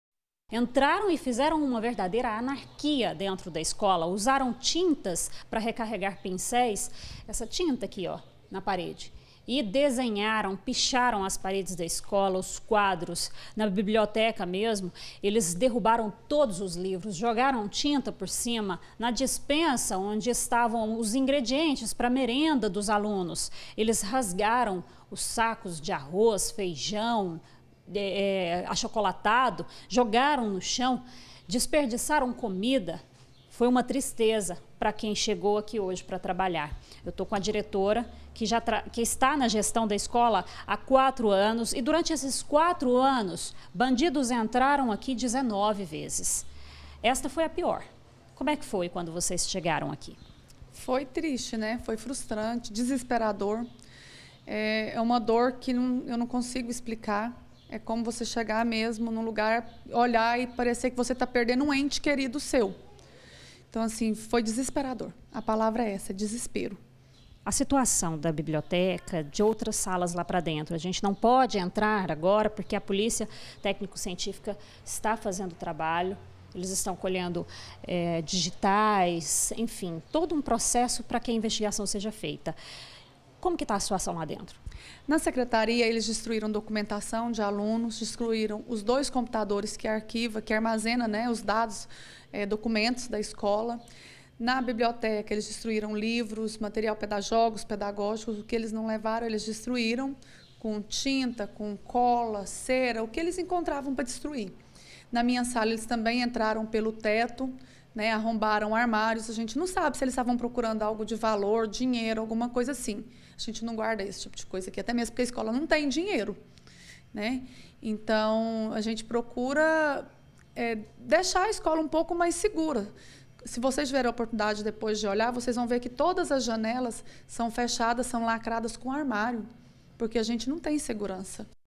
CA - Vandalismo: adolescentes invadem escola e destroem tudo - 23-10-2018
Vândalos entraram em uma escola de Aparecida de Goiânia e destruíram tudo. Salas reviradas, paredes pichadas, eletrodomésticos furtados e até a merenda das crianças foi revirada. E os suspeitos de praticar essa atrocidade são menores que foram apreendidos pela guarda civil metropolitana.